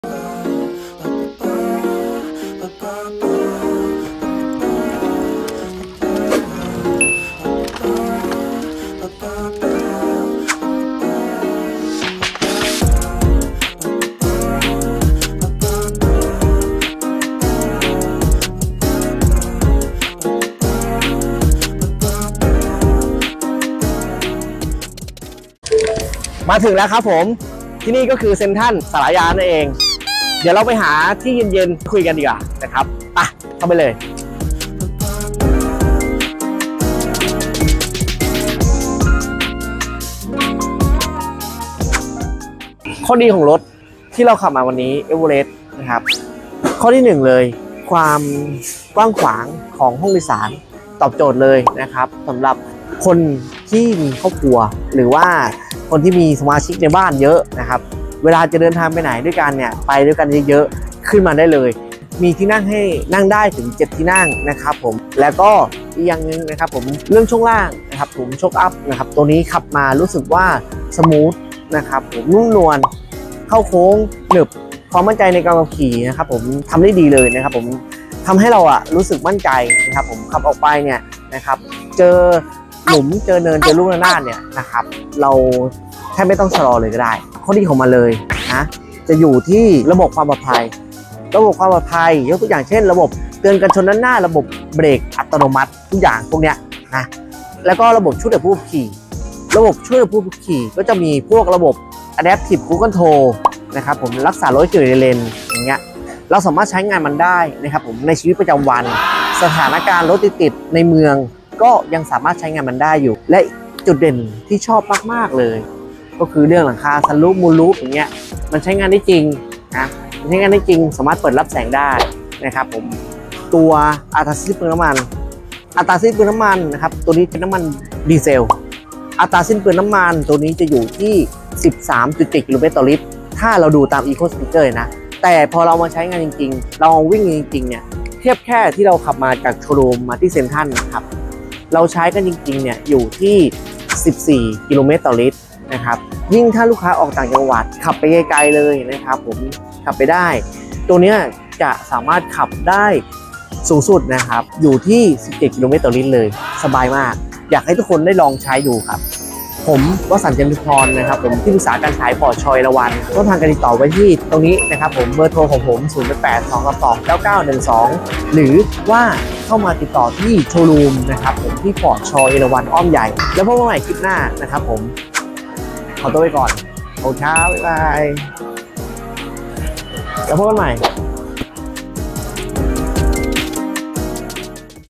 Ford Next-Gen Everest พาเที่ยวใกล้บ้าน
ไปเที่ยวใกล้บ้าน กับฟอร์ด ช.เอราวัณ อ้อมใหญ่ พร้อมข้อดีของFord Everest มีอะไรบ้าง? ไปดูกัน...

ฟอร์ด ช.เอราวัณ อ้อมใหญ่ ให้บริการทั้งฝ่ายขาย และศูนย์บริการ ซ่อมสีตัวถัง ครบวงจร ด้วยช่างมืออาชีพ